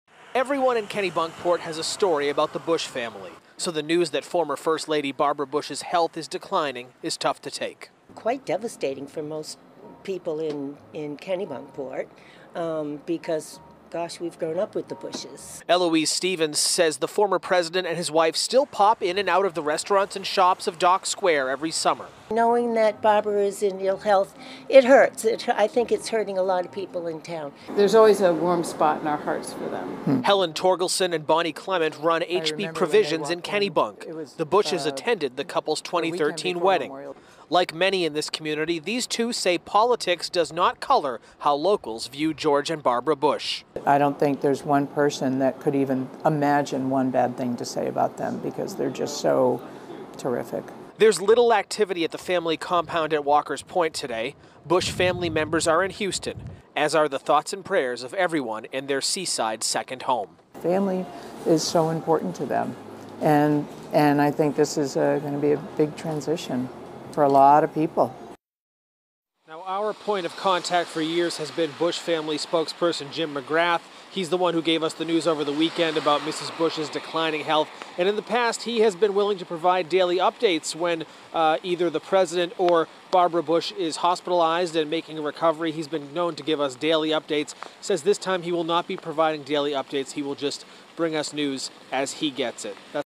'It hurts' Kennebunkport residents to news of Barbara Bush's declining health
Barbara Bush and former President George H.W. Bush have been important parts of the Kennebunkport community for decades. Residents express their sadness after learning about Barbara Bush's declining health.